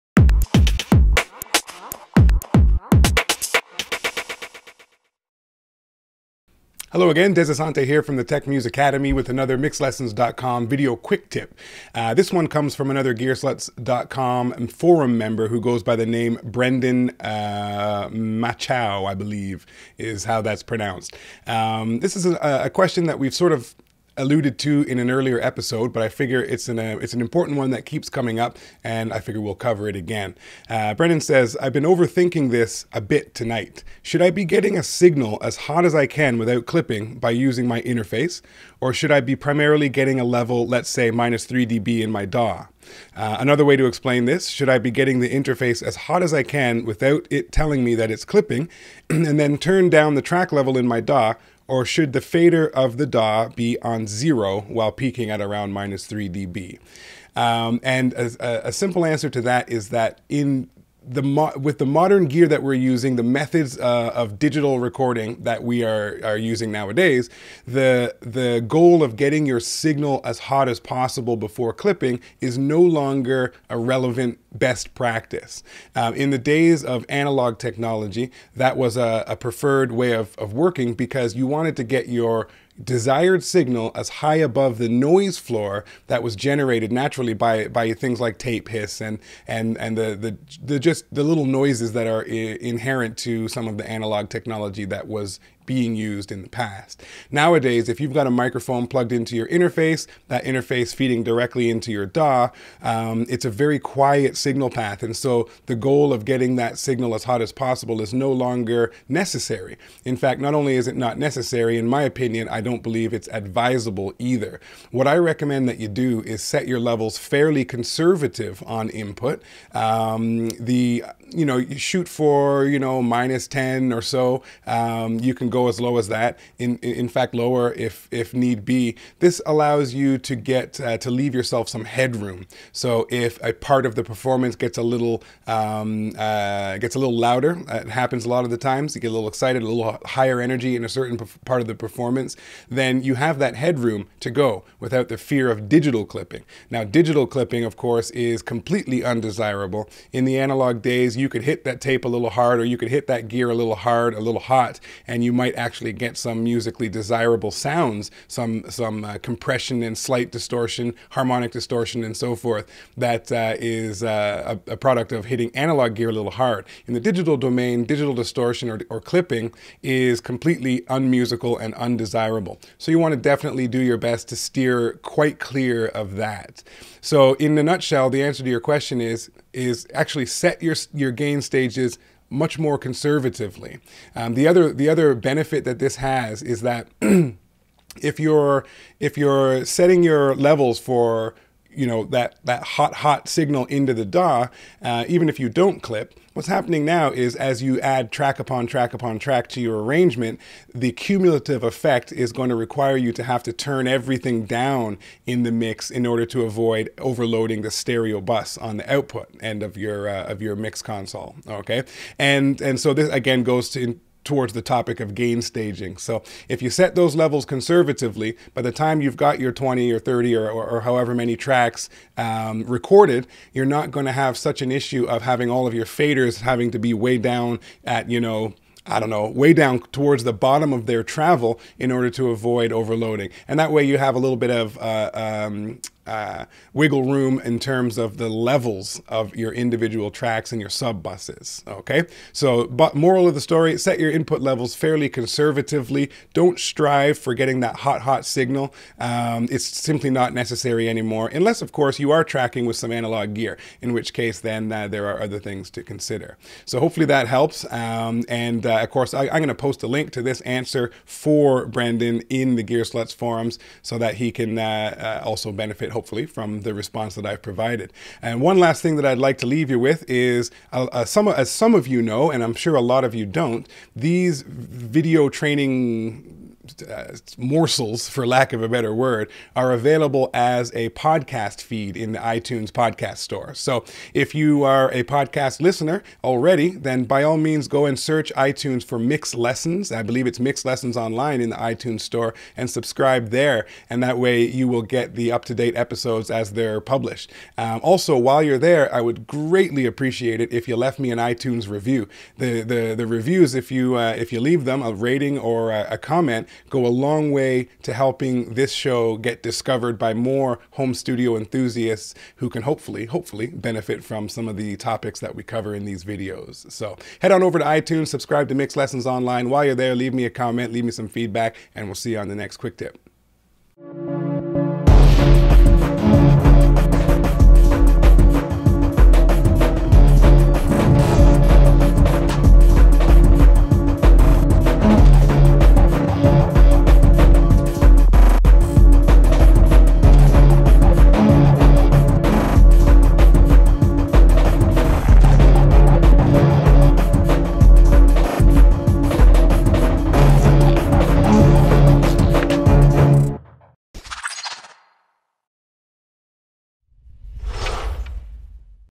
Input Levels Revisited
This is a recap of a topic we've discussed in the past but it's a question that comes up a lot. What is the correct way to set your input gain levels on the way into the computer?

Well, most of the time people try to get the meters as close to zero as possible to achieve the "hottest" signal but is that really the best approach?

These days I typically set my input levels somewhat conservatively. I prefer to leave some headroom for those unanticipated spikes in the performance.

The only potential downside to this is the fact that lower level signals are represented by fewer digital bits. So, don't be too conservative but leave yourself some breathing room!